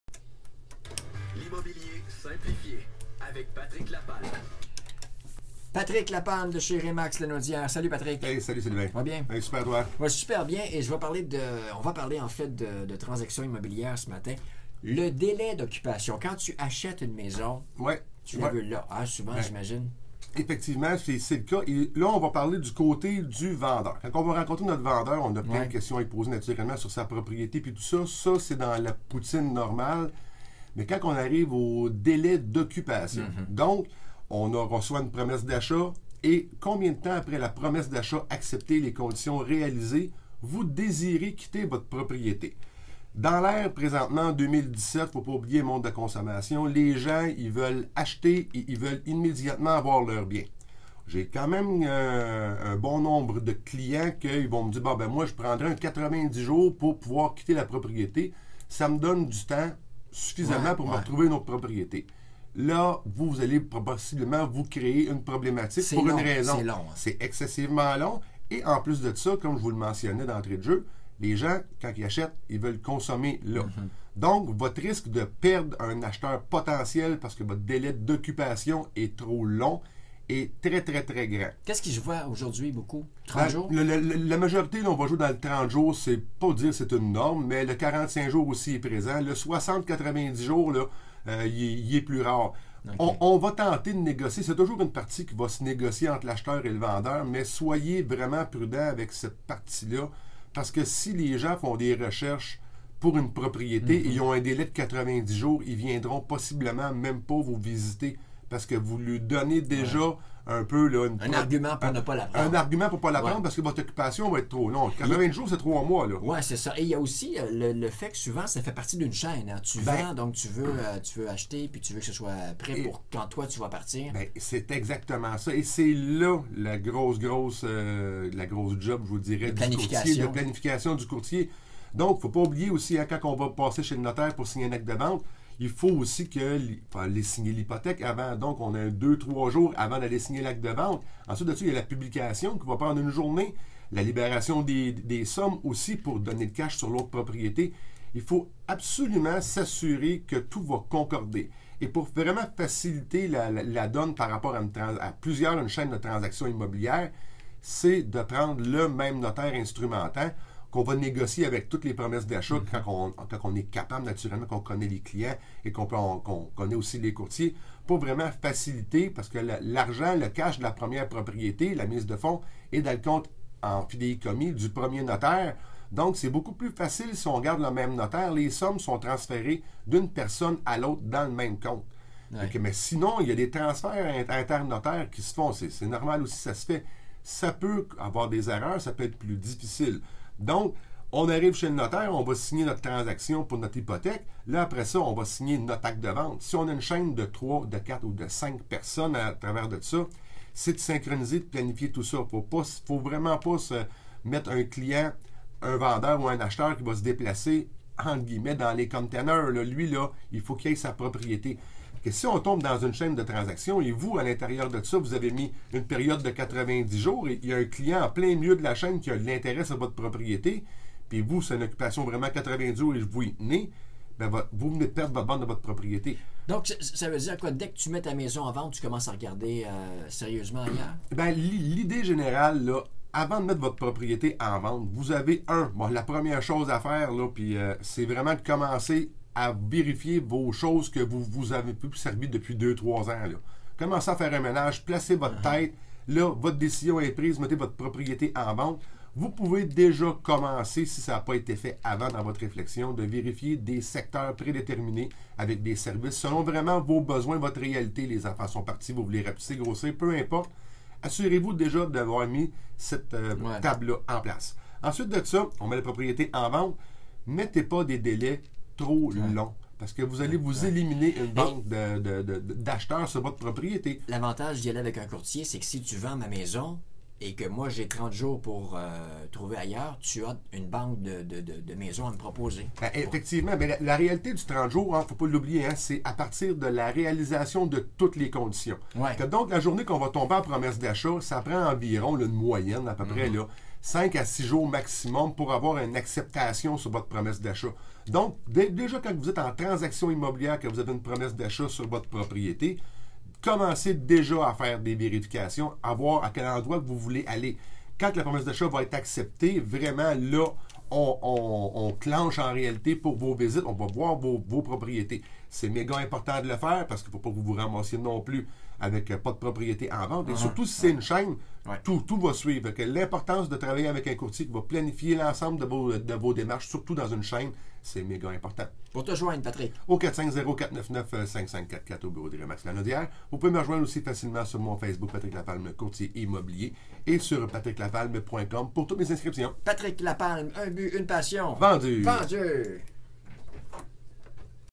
Patrick Lapalme Dates d'occupations et les chaînes de transactions M 103,5 24 10 2017
Voilà ma chronique de cette semaine –  Dates d'occupations et les chaînes de transactions - avec Sylvain Morin au M103,5FM - Radio de Lanaudière! Bon visionnement ! CLIQUEZ J'AIME ET PARTAGEZ !!! Si vous avez des commentaires ou questions sur mes chroniques, n'hésitez pas!